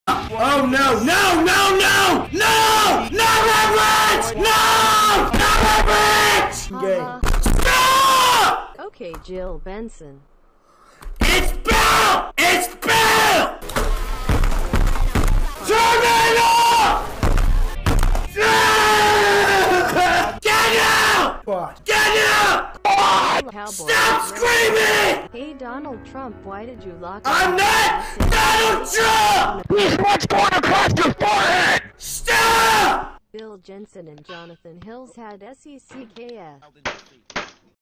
Fishing spot reviewer and nickelpickle lip sync Mock bill Jenson (Edit)
this video is let Stephen know that he's a good guy and I'm on your side. especially when it comes to Bill Jensen wanting to box Stephen quire. Stephen, I wish you the best of luck and looking forward to meeting you one day.